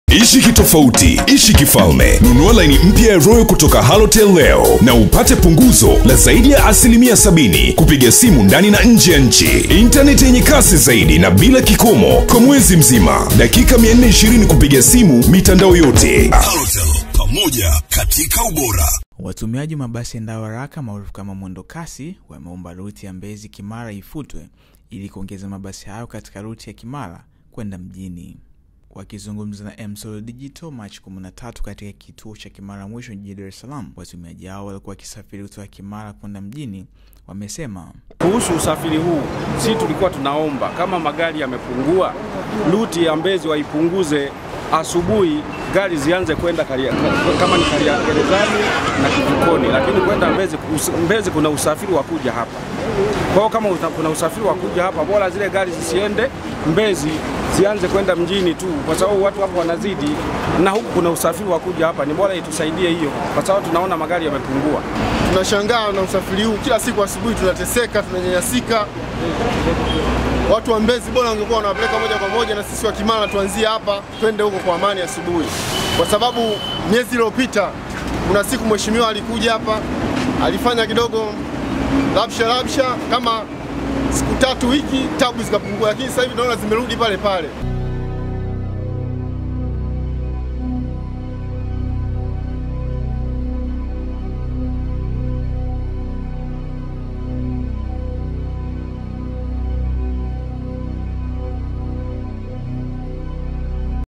WATUMIAJI WA MABASI YAENDAYO HARAKA (UDART) WAOMBA RUTI YA MBEZI – KIMARA IFUTWE
Watumiaji wa Mabasi Yaendayo Haraka maarufu kama mwendokasi (Udart) wameomba ruti ya Mbezi – Kimara ifutwe ilikuongeza mabasi hayo katika ruti ya kimara kwenda mjini.